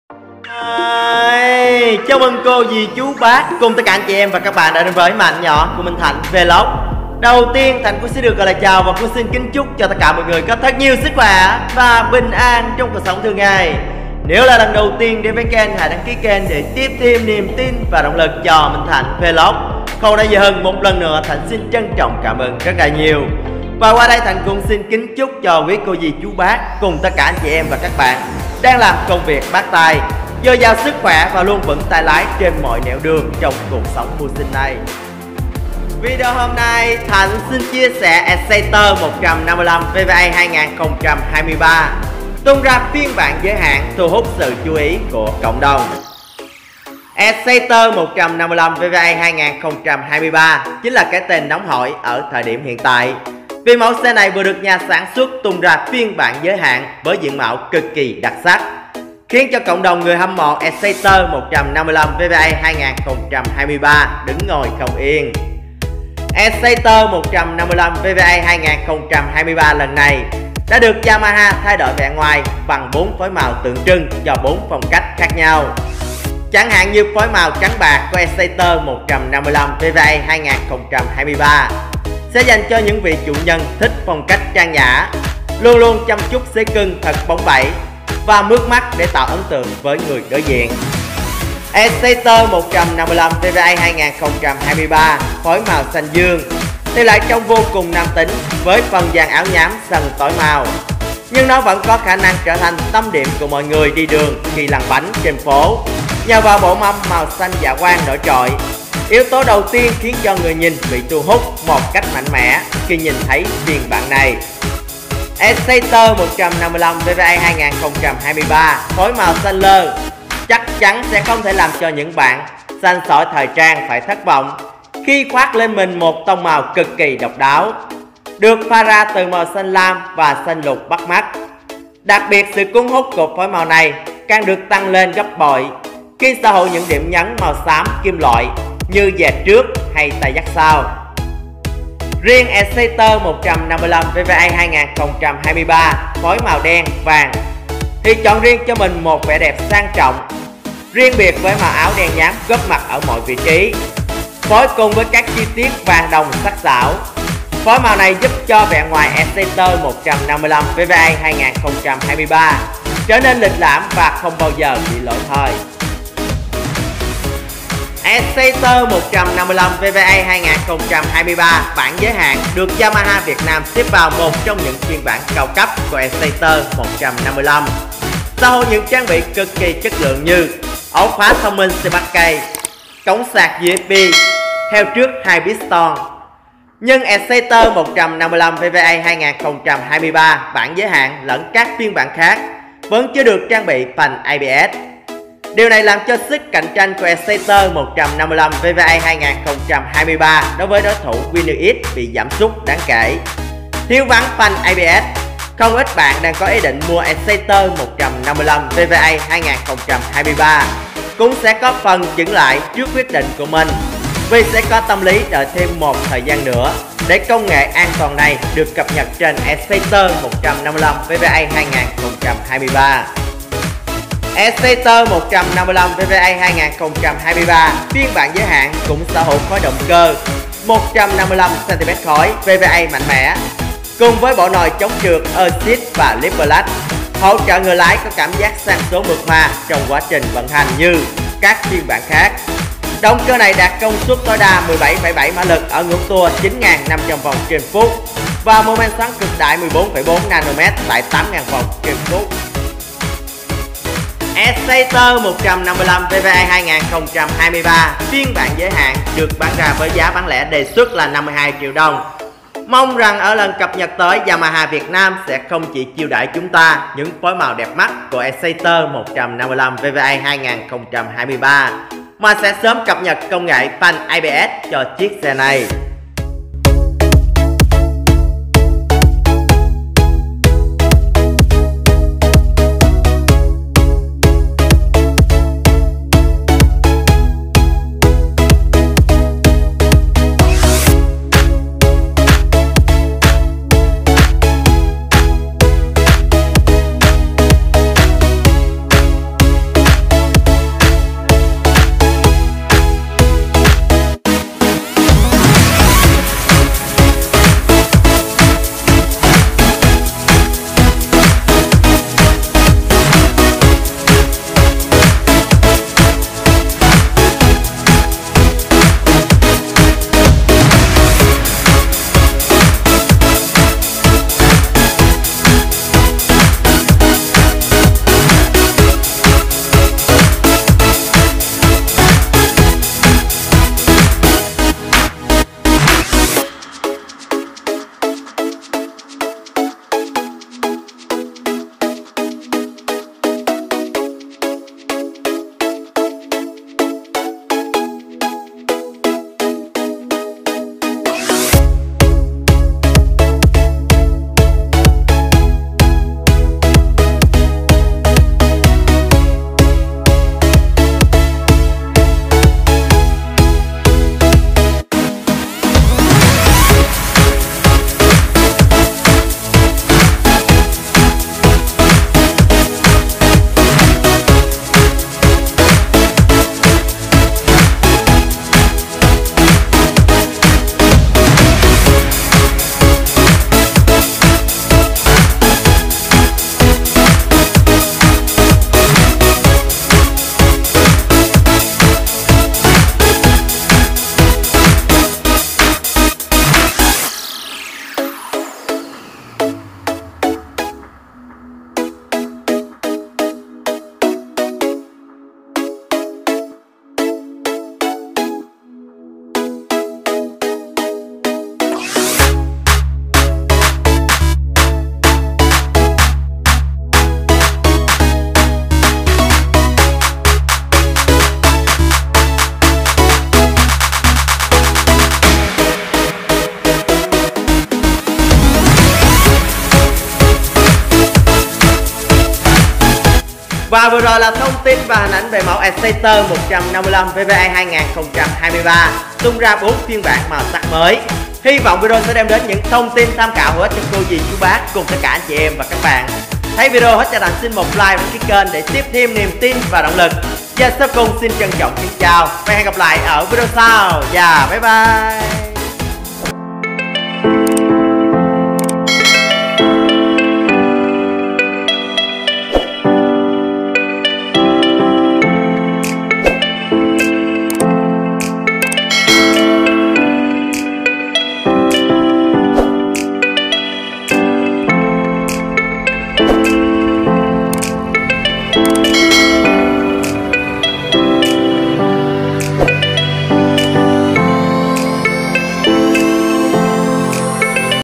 yamaha exciter 155vva 2023: chi tiết tem các phiên bản giới hạn có gì?
exciter tem các phiên bản giới hạn như thế nào?